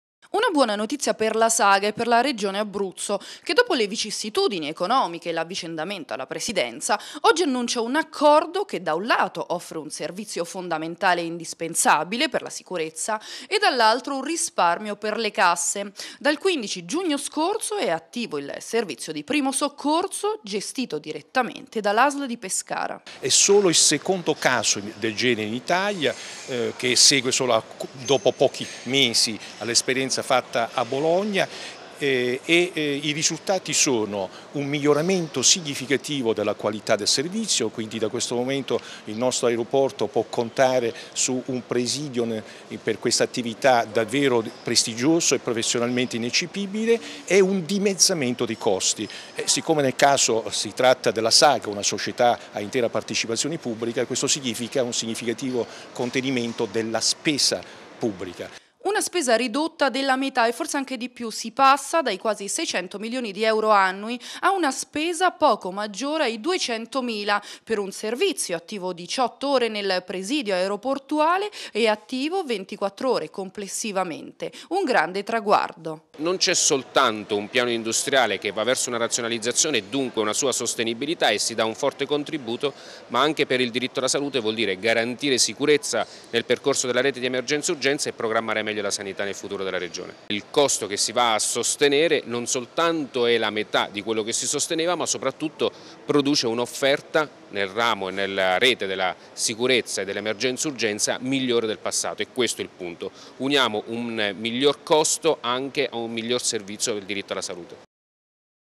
Aeroporto d'Abruzzo Torna il presidio 118
La Saga stringe un nuovo accordo per ripristinare il presidio di primo soccorso all interno dell ente aeroportuale, sarà la Asl a gestire direttamente il servizio con un dimezzamento delle spese